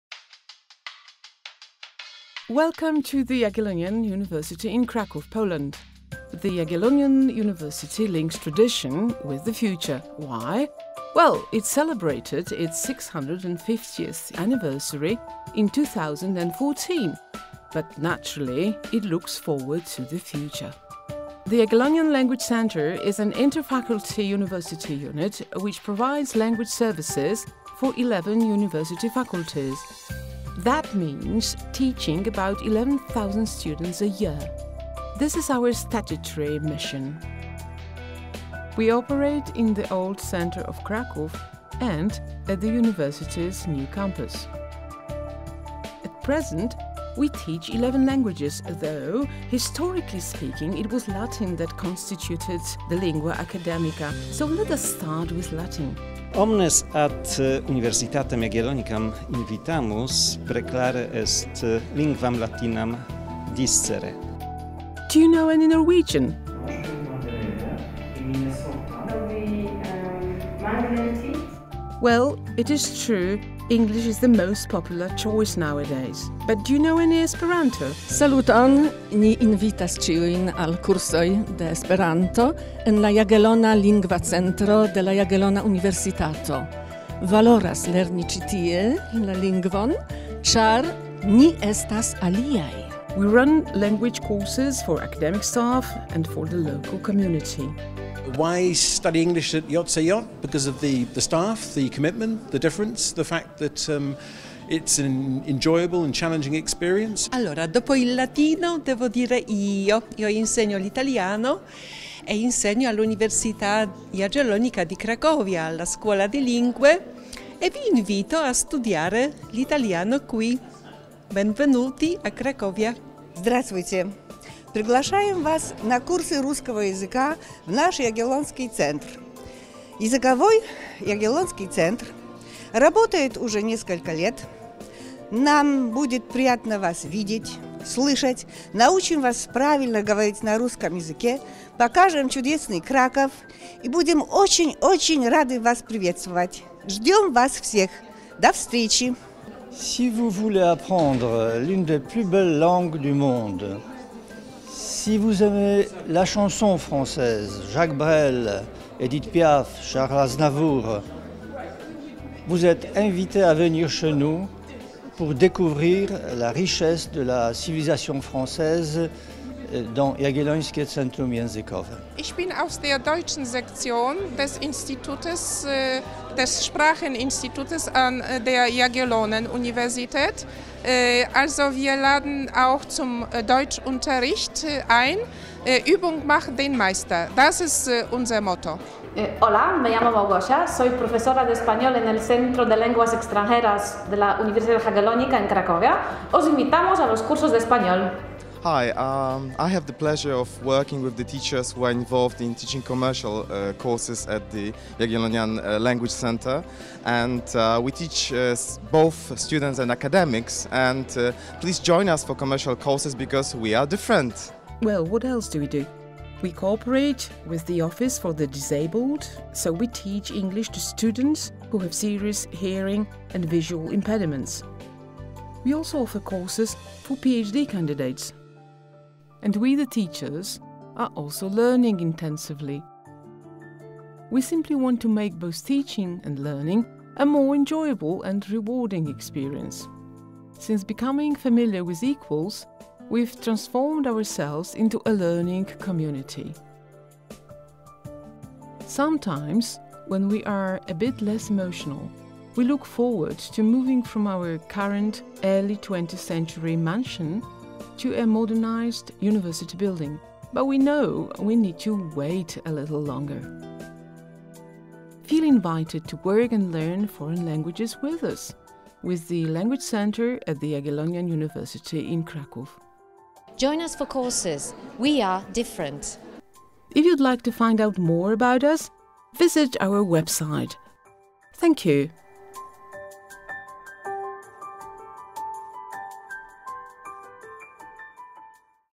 Jagiellońskie Centrum Językowe - prezentacja
Uczymy języka angielskiego, francuskiego, niemieckiego, włoskiego, hiszpańskiego, rosyjskiego, ale także litewskiego, chińskiego oraz łacińskiego.